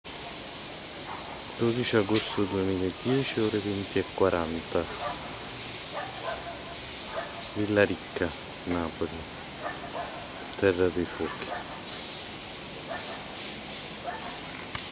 12 Agosto 2010 ore 20.40
NON SONO SOLO I CAMPI ROM, ma soprattutto quelli. Anche in Grecia ricavavano rame bruciando i pneumatici